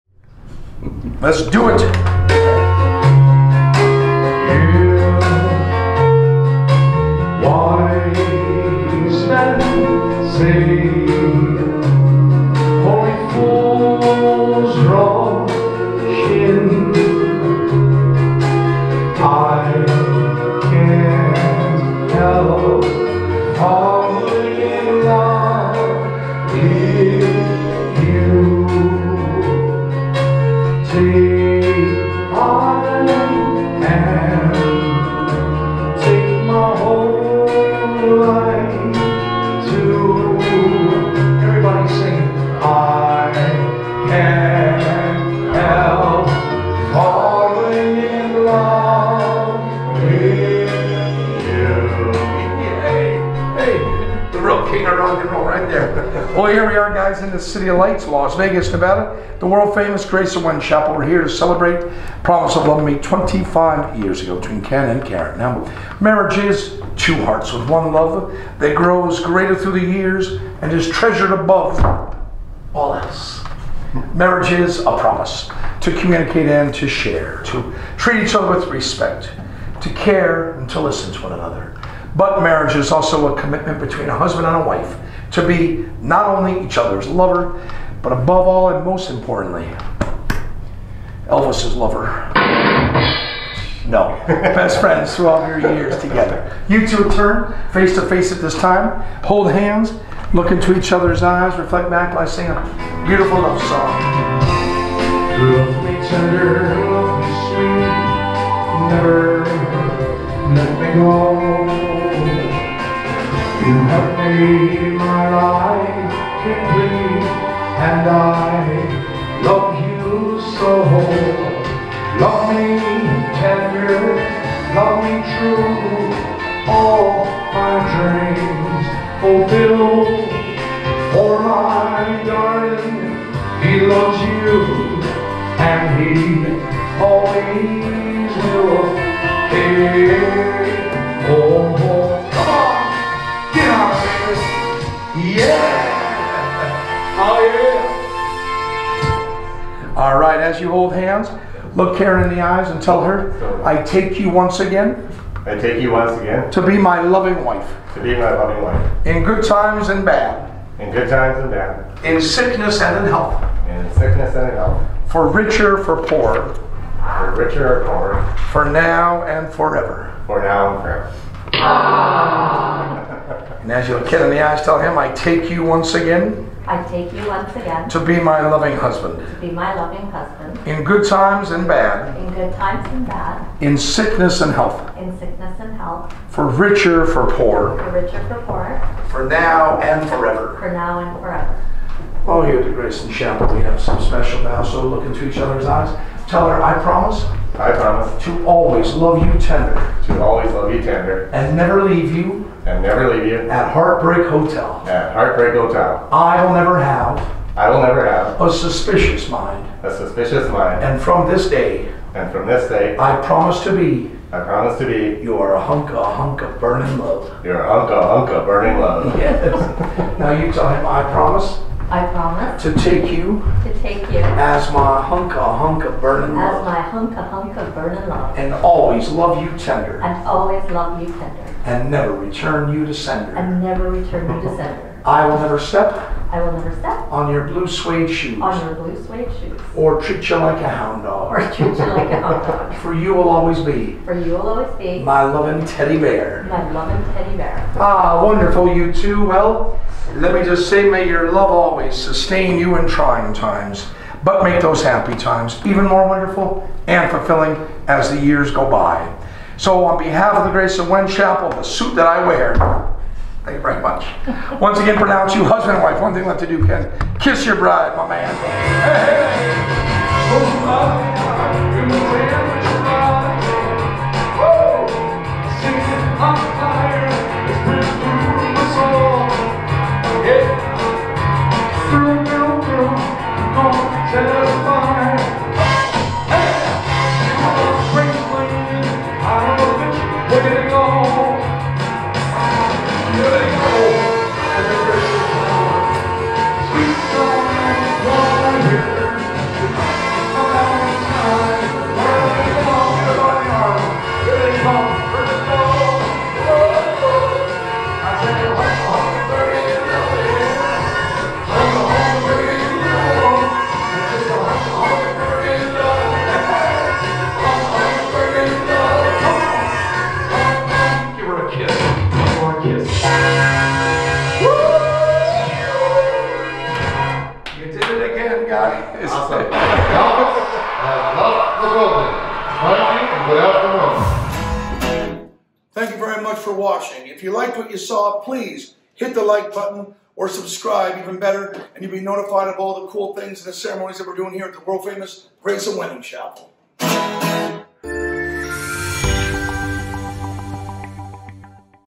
Ken & Karen's Elvis Vow Renewal in Las Vegas | Graceland Chapel
Ken & Karen's Elvis Vow Renewal Ceremony at Graceland Chapel in Las Vegas Nevada.

Happy Anniversary!

Home of the World's First Elvis Themed Ceremony!